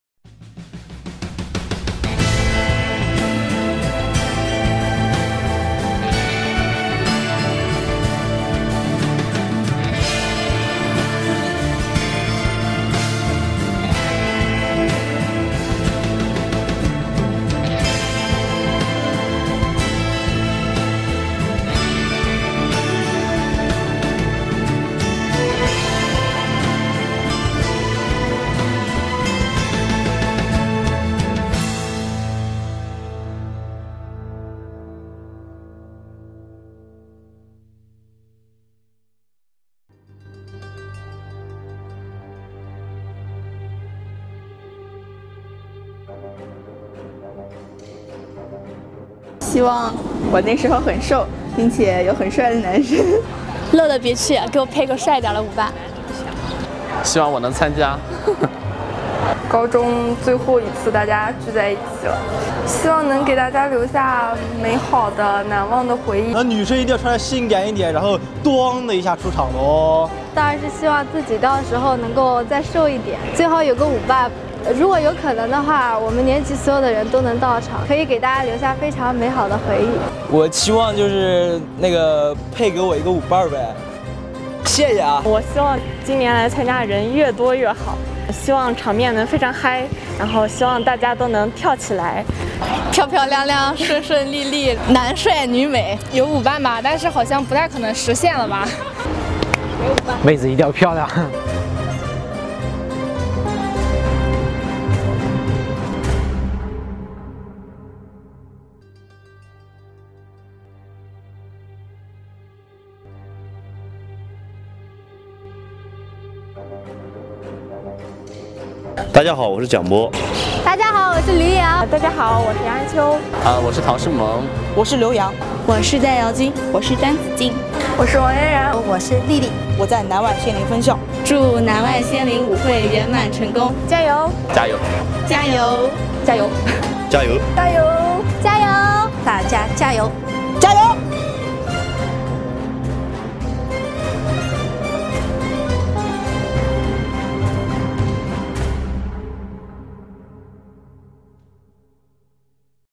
Prom Prevue of Nanjing Foreign Language School, Xianlin Campus.
Senior Prom hosted by few of students who once went to American as exchange students~ Prom will be located in Sofitel Zhongshan Golf Resort.